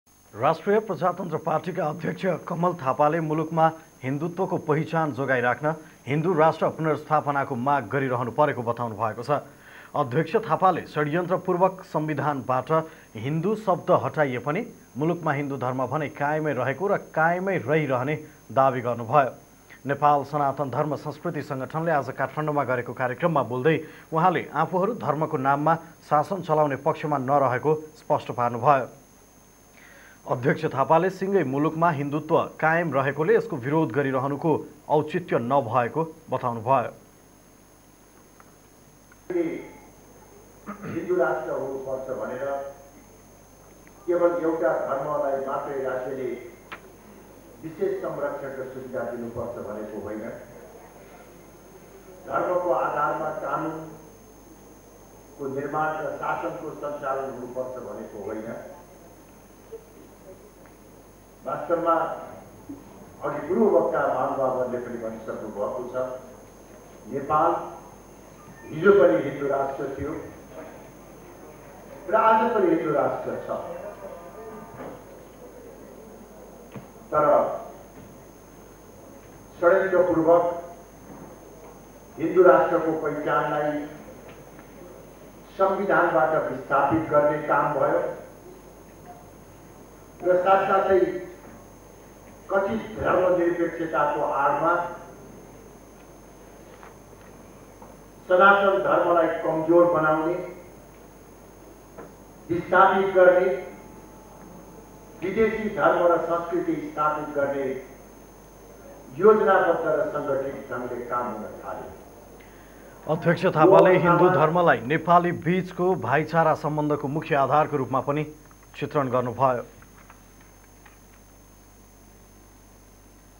हिन्दुत्वको पहिचान जोगाई राख्न हिन्दु राष्ट्र पुनस्र्थापनाको माग
राष्ट्रिय प्रजातन्त्र पार्टीका अध्यक्ष कमल थापाले मुलुकमा हिन्दुत्वको पहिचान जोगाई राख्न हिन्दु राष्ट्र पुनस्र्थापनाको माग गरिरहनु परेको बताउनु भएको छ ।